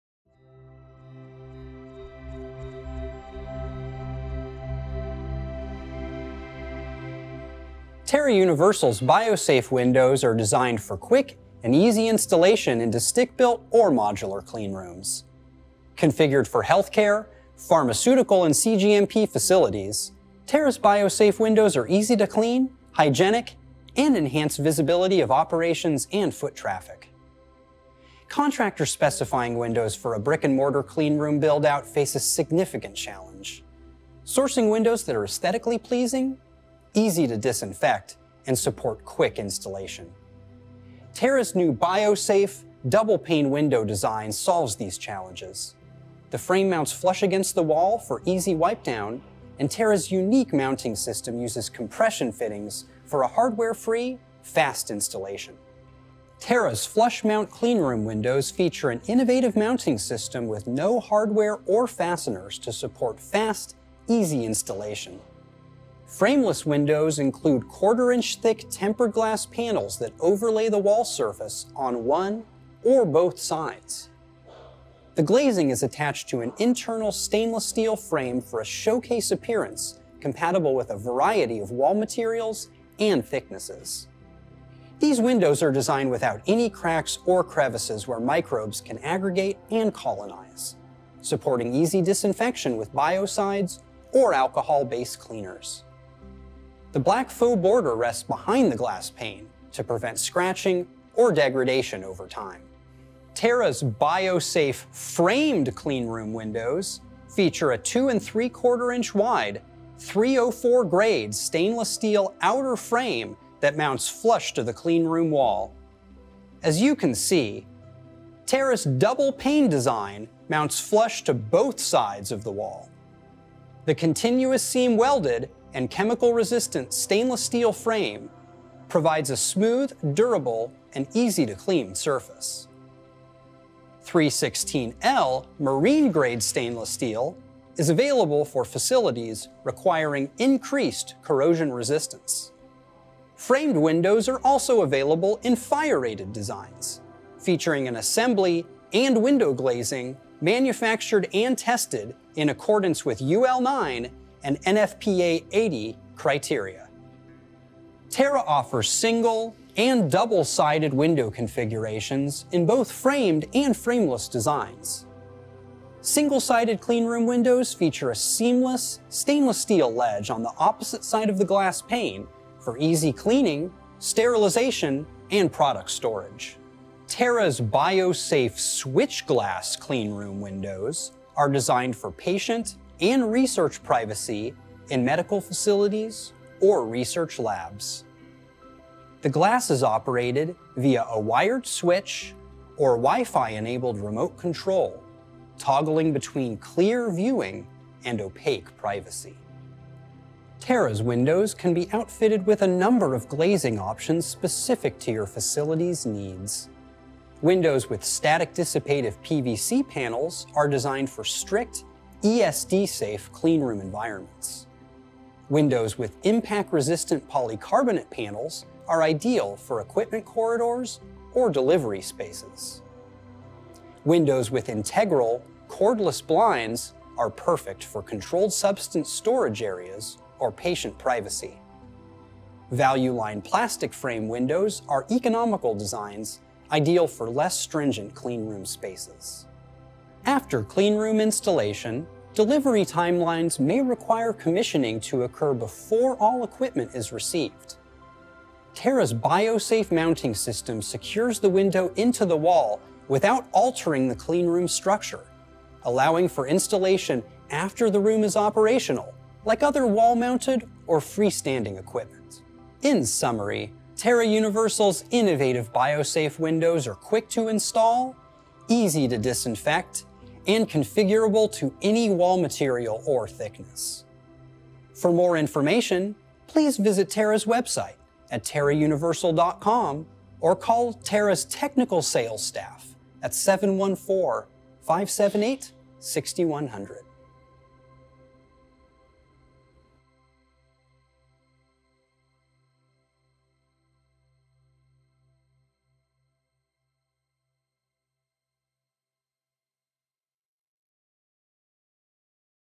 Terra Universal's Biosafe® Windows
Terra Universal's BioSafe® windows are quick to install, easy to disinfect and configurable to any wall material or thickness.